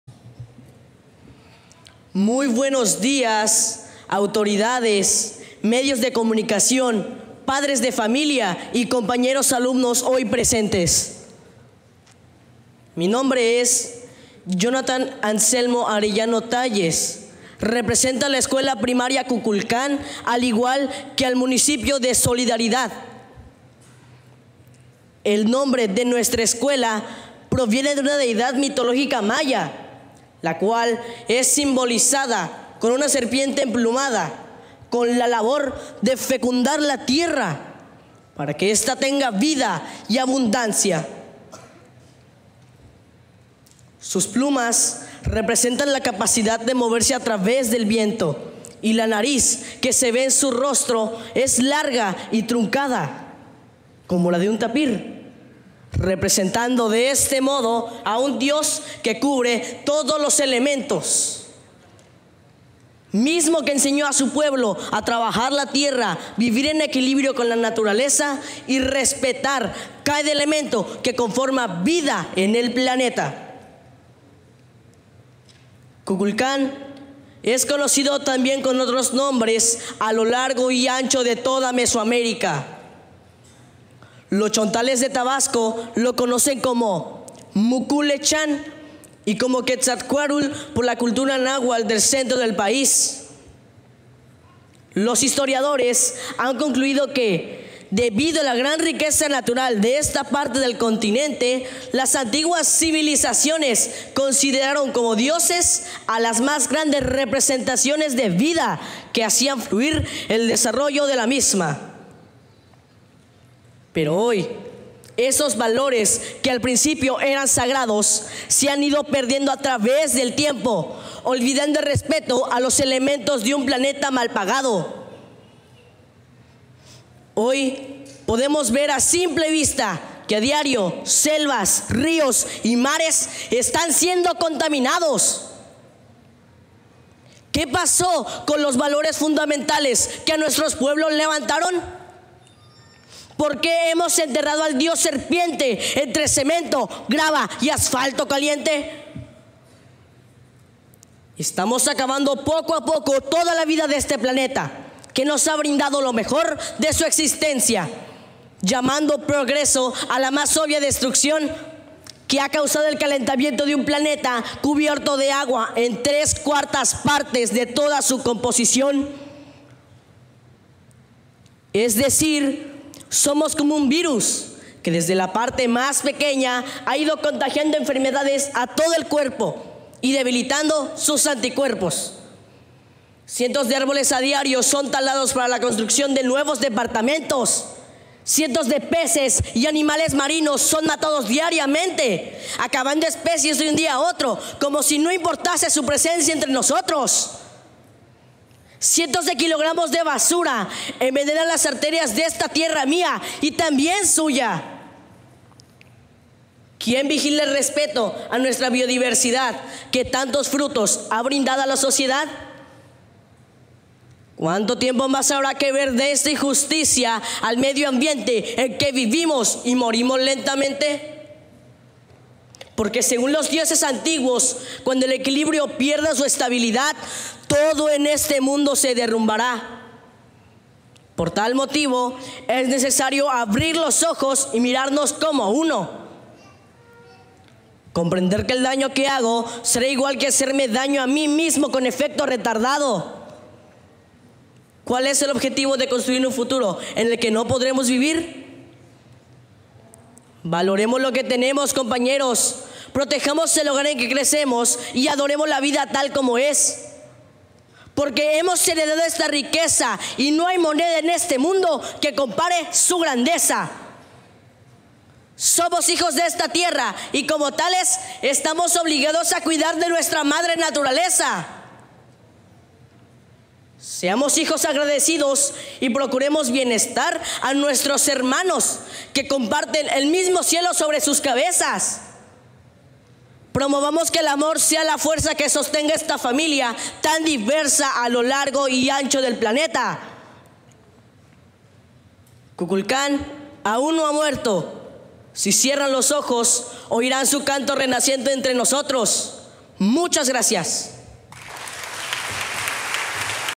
Intervención del Dip. Infantil Jhonatan A. Arellano Talles. Municipio de Solidaridad (25/Abr/2018)
Intervención del niño Jhonatan Ancelmo Arellano Talles diputado infantil del Municipio de Solidaridad.

Tema: Cuidado del medio ambiente y la biodiversidad.